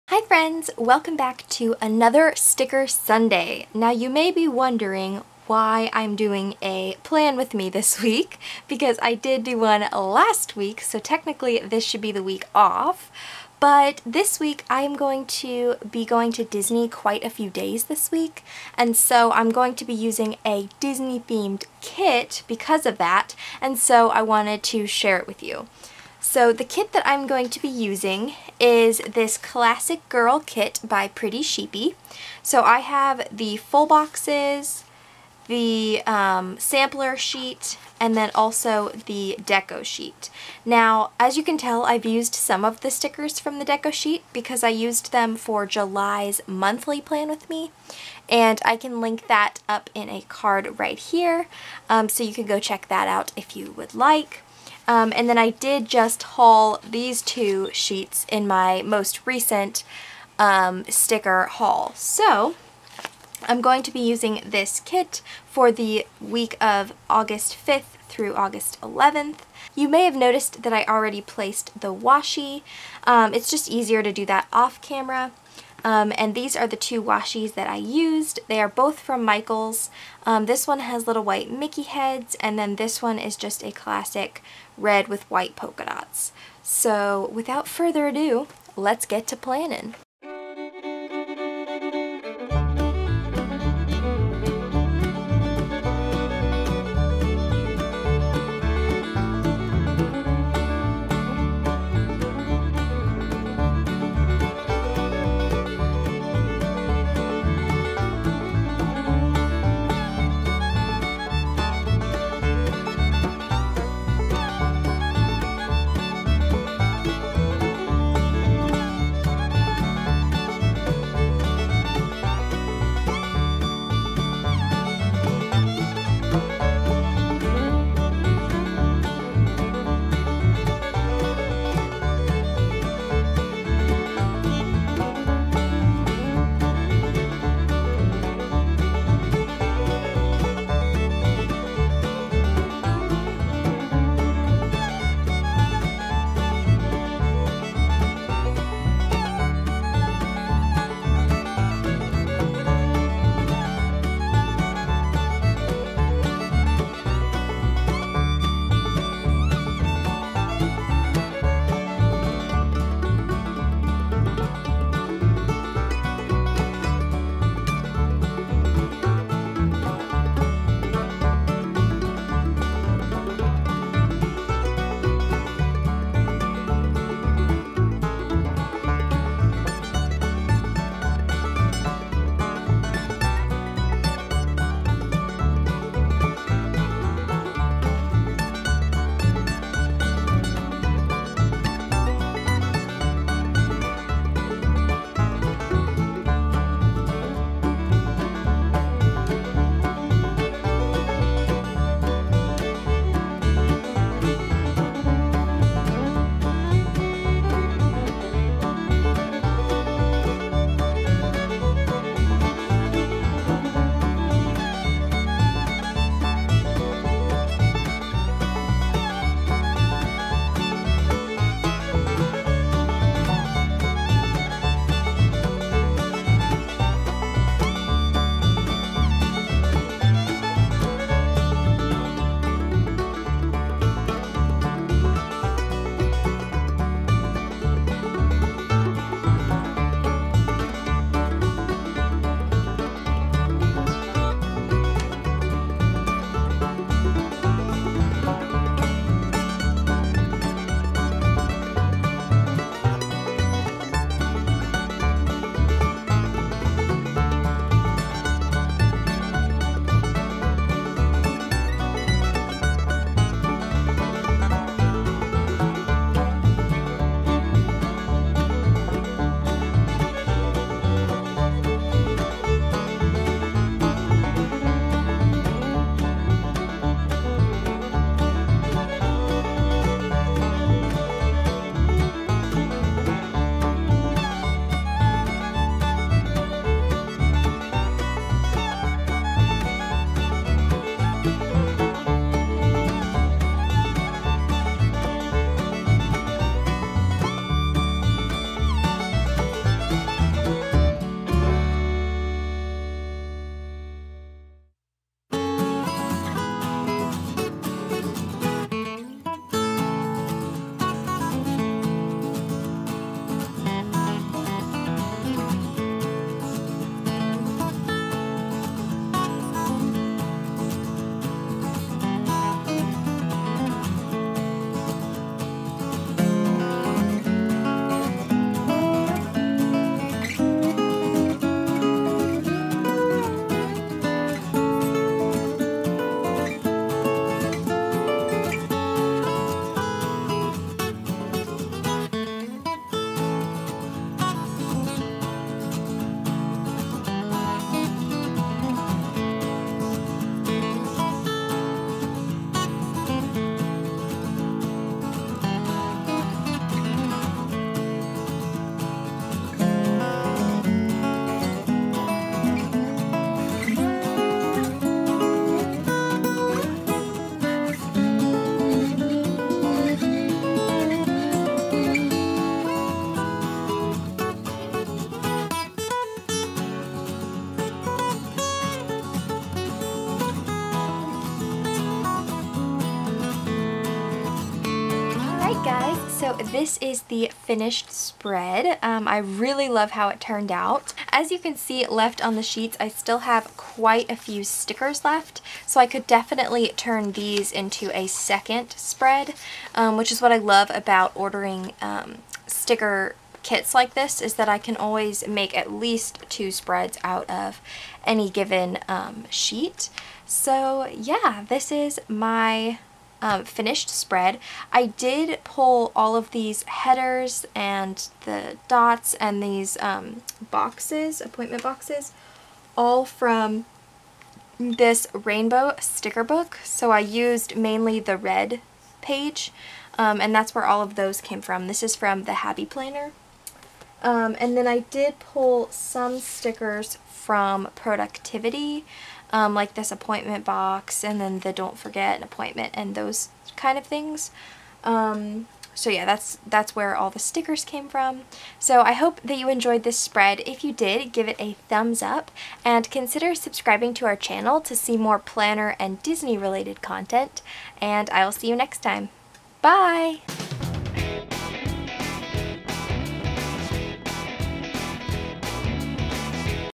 Disney Inspired Plan with Me featuring Pretty Sheepy - Sticker Sundays - TheDisneySisters
In today's Sticker Sunday video, I share a bonus plan with me featuring the "Classic Girl" stickers from Pretty Sheepy!

If you'd like to send us a postcard or mail, we'd love to hear from you!